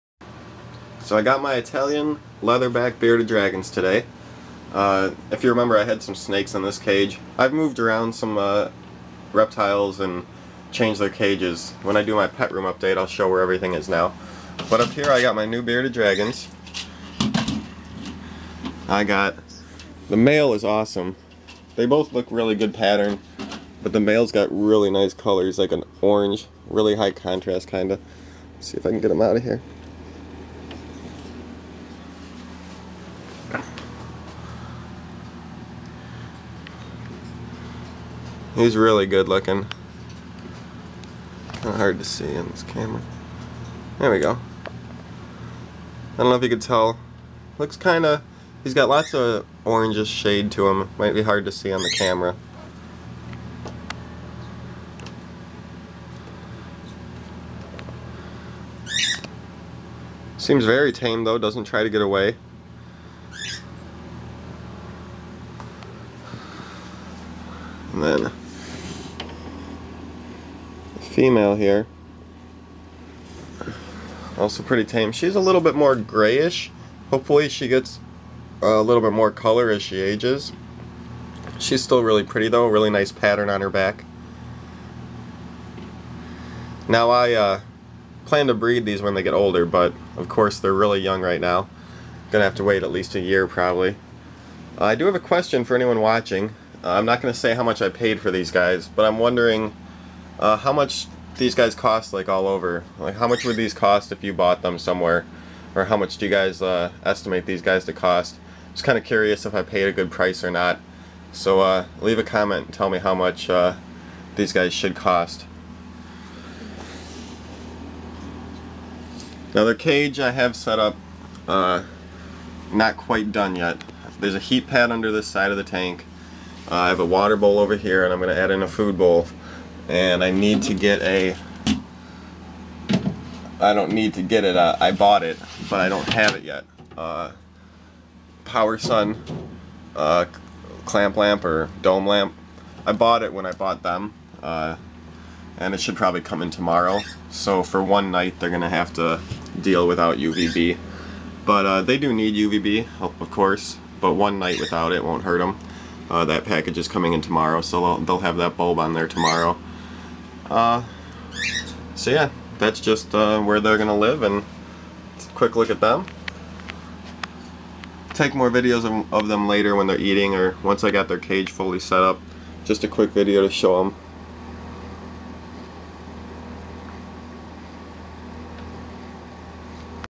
Rare Leatherback Bearded Dragons!
My pair of leatherback bearded dragons!  Just received them Aug. 12, 2009!  Great looking pair!